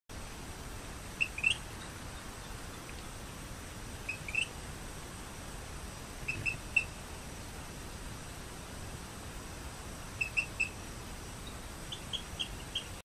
Eleutherodactylus cystignathoides (Rio Grande Chirping Frog)
Image taken in Broussard, Lafayette Parish, Louisiana 2 July 2010.  Voice recording also obtained from Broussard 14 August 2012.